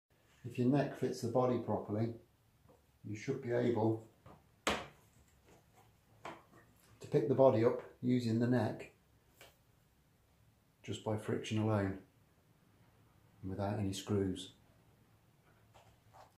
How a guitar neck should fit...
A quick demonstration of what a properly fitting guitar neck looks like.